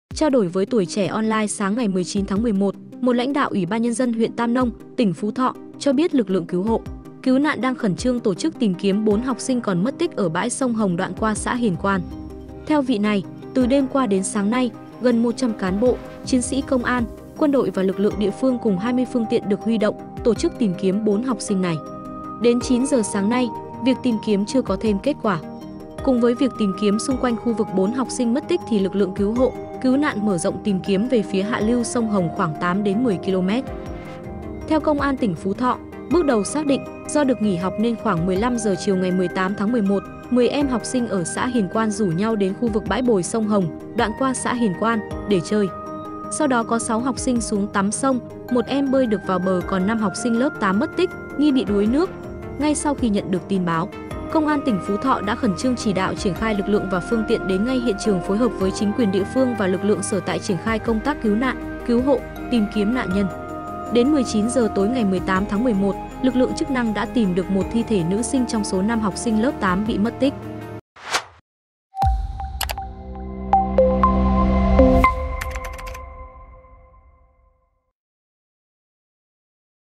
Vụ 5 học sinh mất tích ở bãi sông Hồng: Mở rộng tìm kiếm 10km về hạ lưu
Tỉnh Phú Thọ huy động gần 100 người cùng 20 phương tiện, xuyên đêm tổ chức tìm kiếm 4 học sinh còn mất tích ở bãi sông Hồng, đoạn qua xã Hiền Quan, huyện Tam Nông.